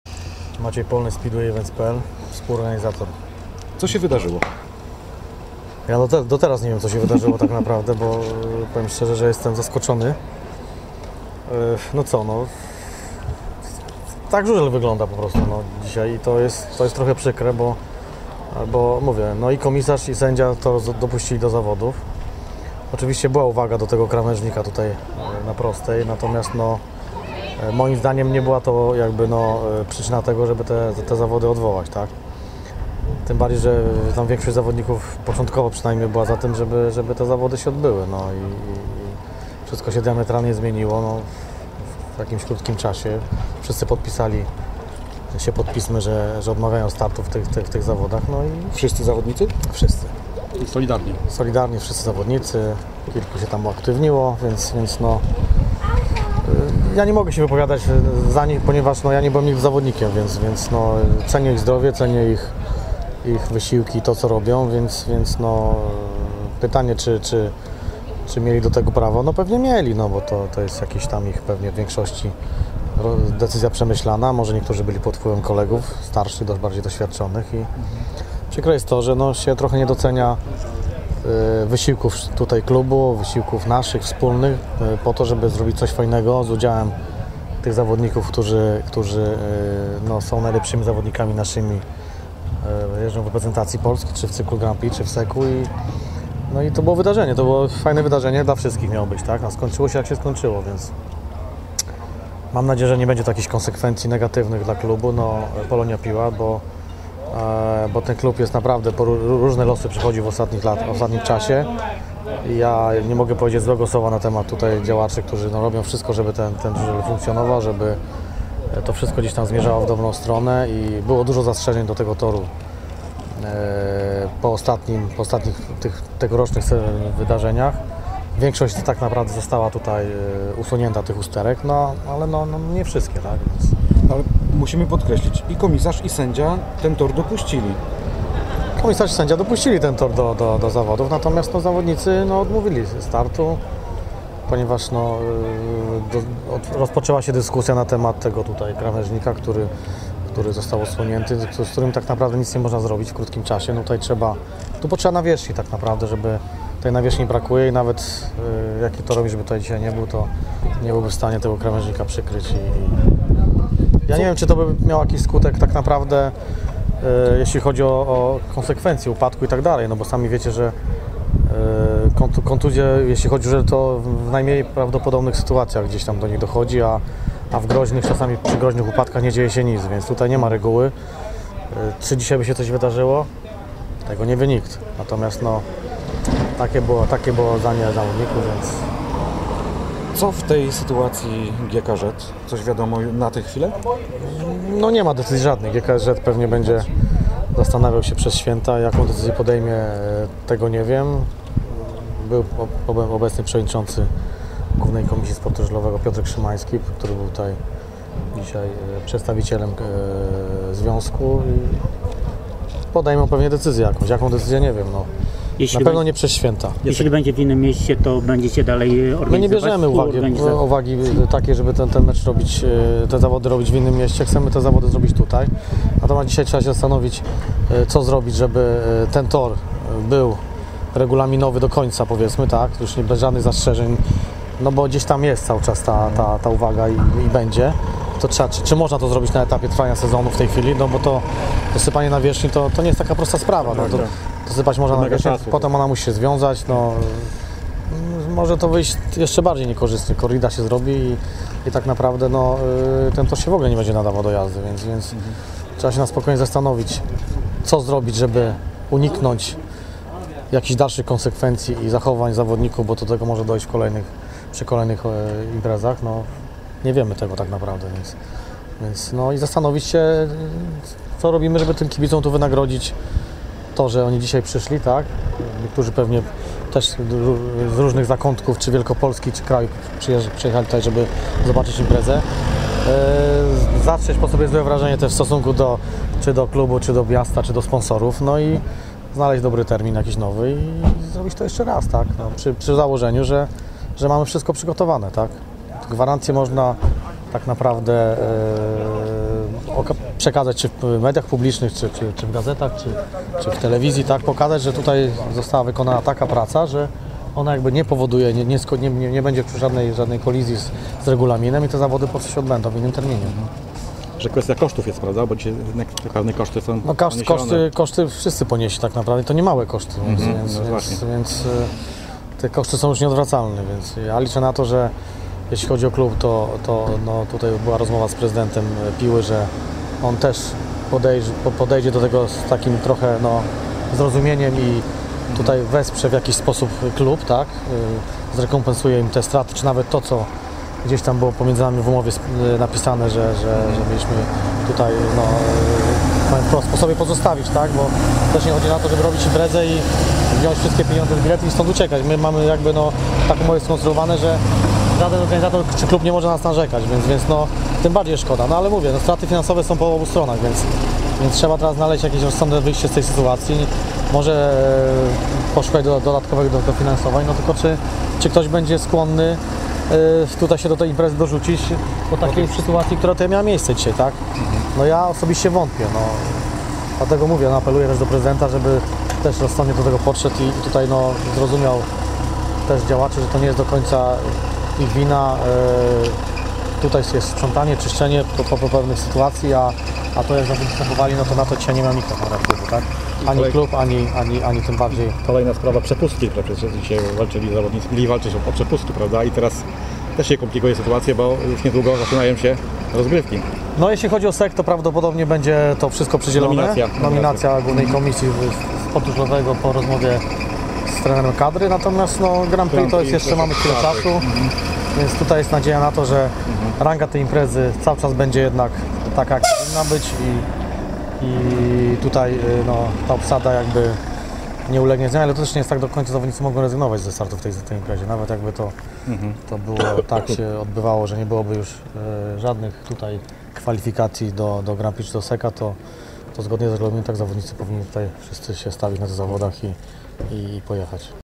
Złoty Kask w Pile odwołany- M. Polny
Maciej  Polny  z firmy SpeedwayEvents.pl, która była współorganizatorem zawodów.